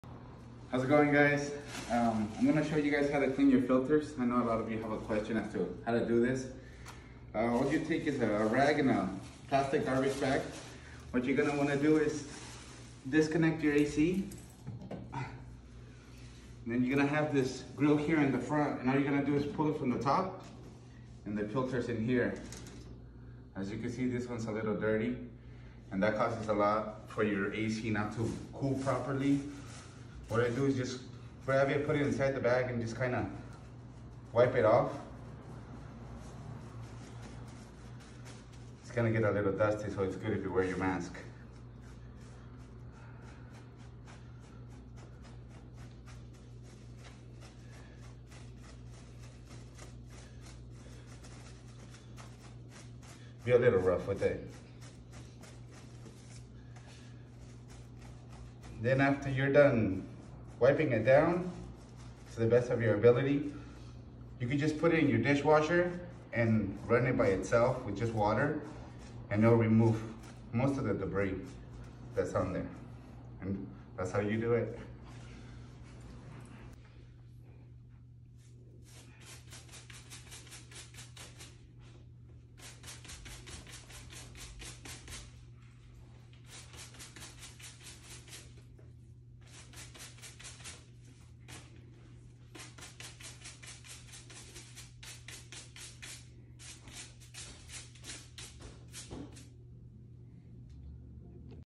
Maintenance Tips at Nexus!
Tips and tricks directly from the Nexus service team!  Roberto is our maintenance supervisor and is sharing the easiest way to clean your PTAC filter.